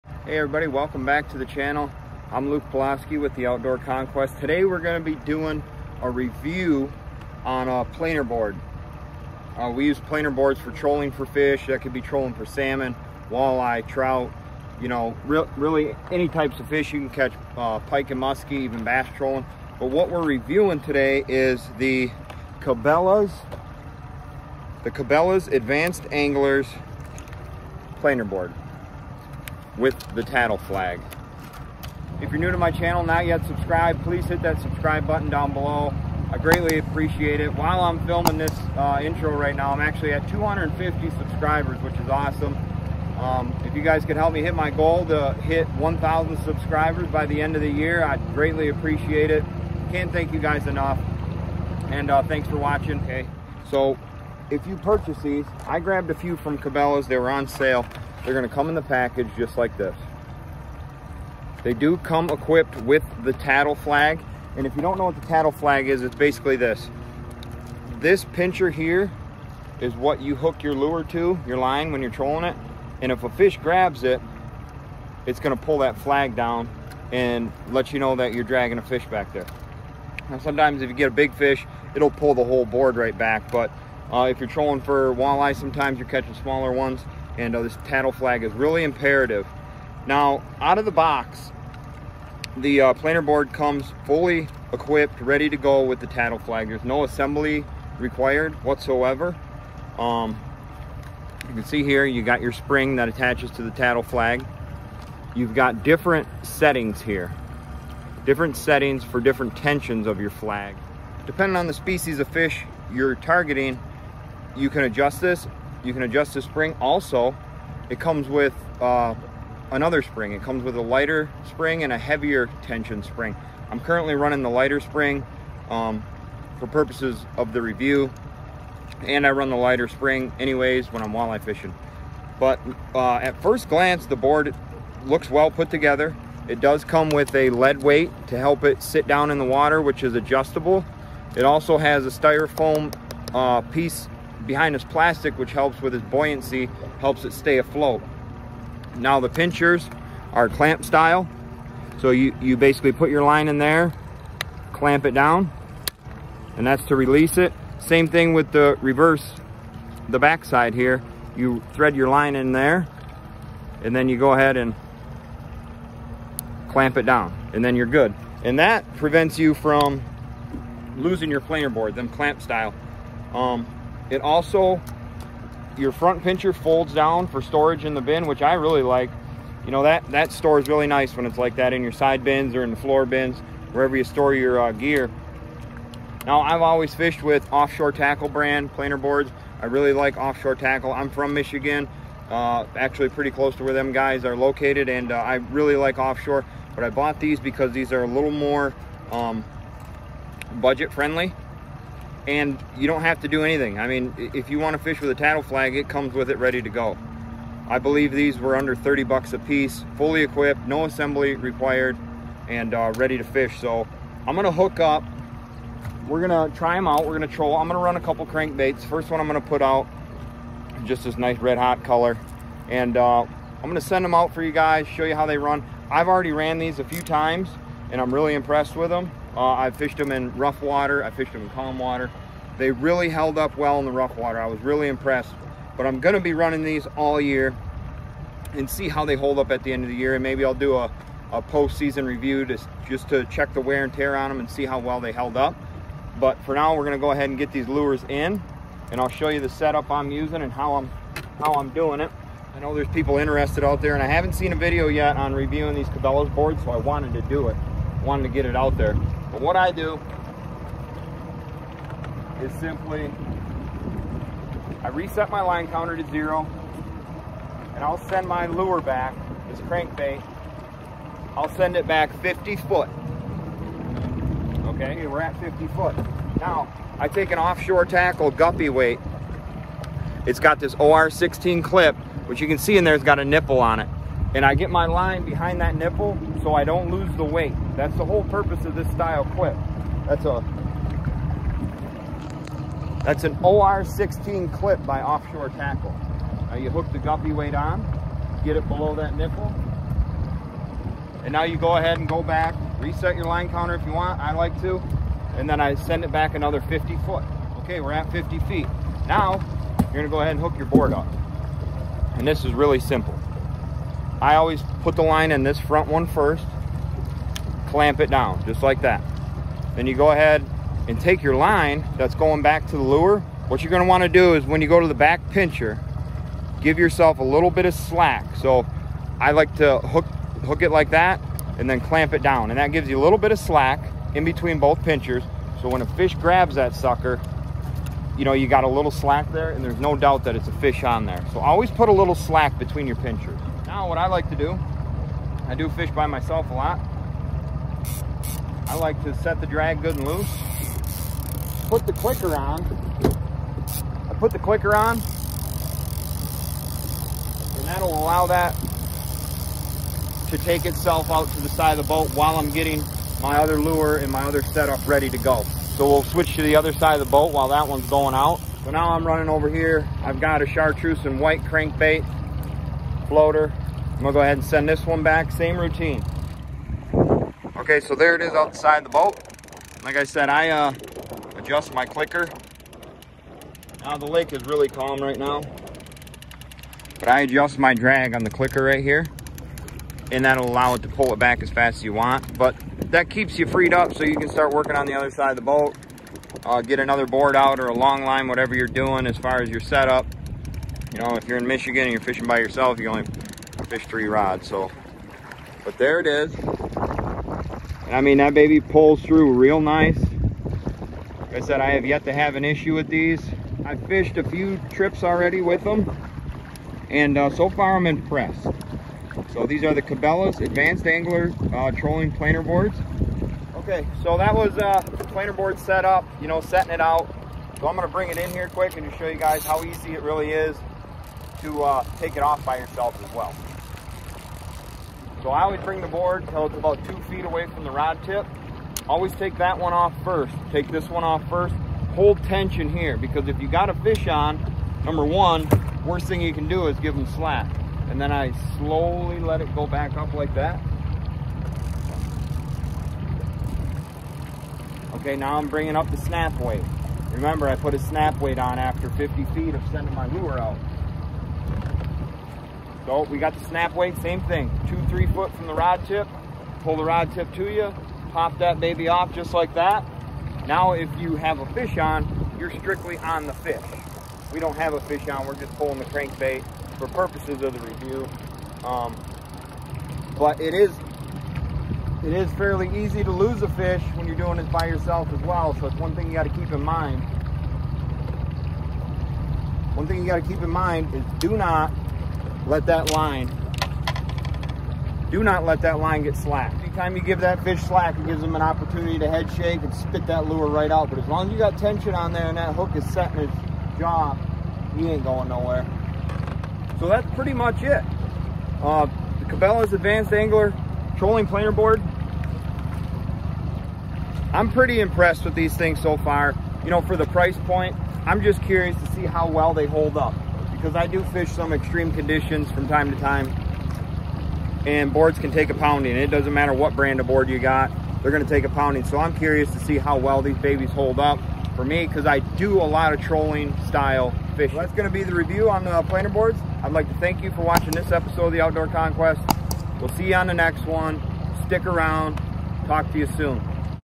How to Use Planer Boards | Cabelas Advanced Angler Planer Board Review |Trolling with Planer Boards
In this video I review the Cabelas Advanced Angler side planer board. So far I’m very impressed. I will continue to update you all with their performance and durability.